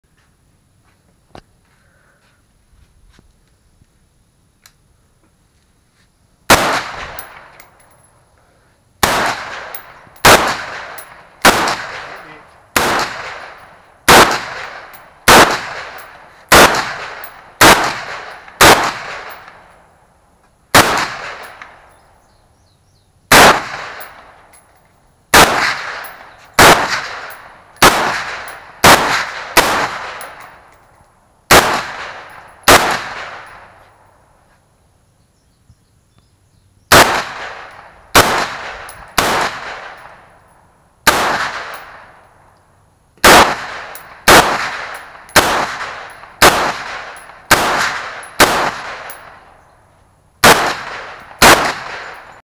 Shooting my AR 8-1-15
Shooting my AR 223/5.56 At Sandusky County Sportsmans club.